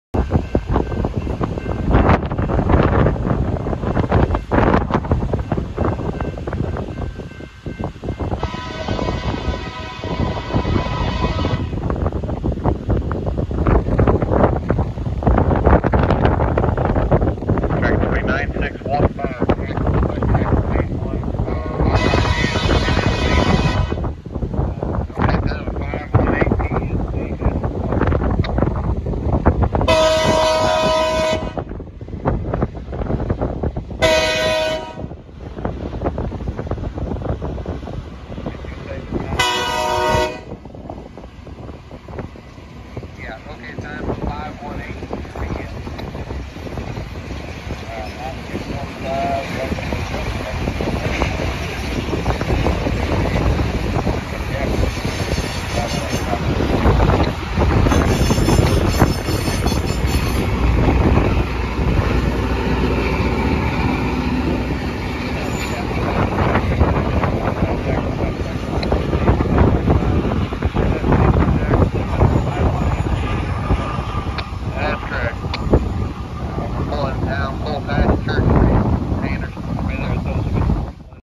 Loram Rail Grinder headed West to stop at Jacksonville, IL
Loram Rail Grinder came through to tie down in Jacksonville on the interlocking track, wasn't grinding unfortunately but still cool to see.

Do NOT use my videos without my permission!